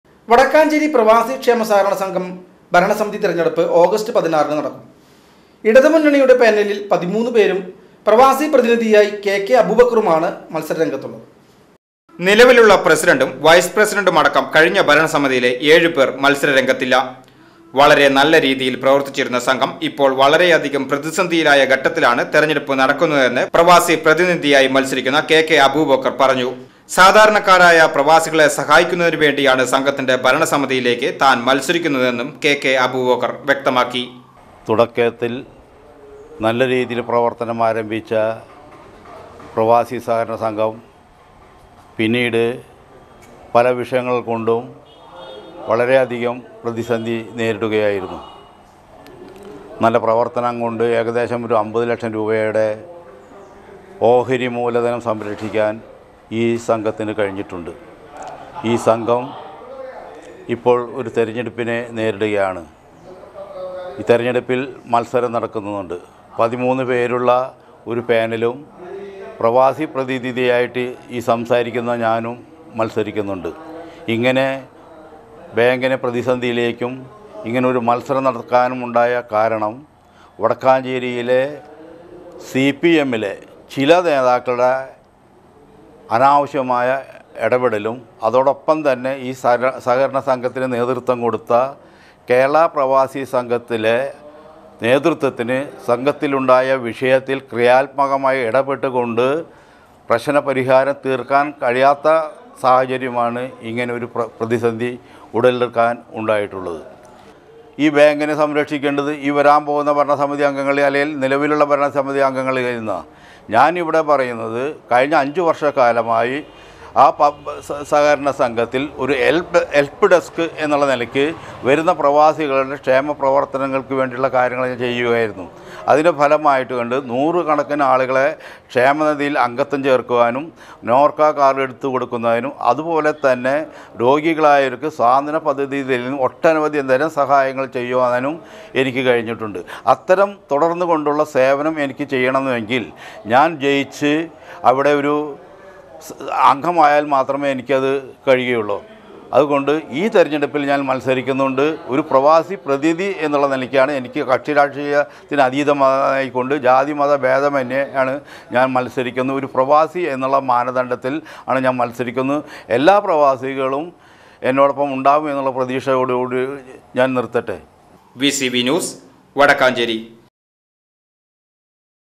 വടക്കാഞ്ചേരി പ്രവാസി ക്ഷേമ സഹകരണ സംഘം ഭരണസമിതി തെരഞ്ഞെടുപ്പ് ഓഗസ്റ്റ് 16ന് നടക്കും
VCV NEWS

Wadakkanchery, Thrissur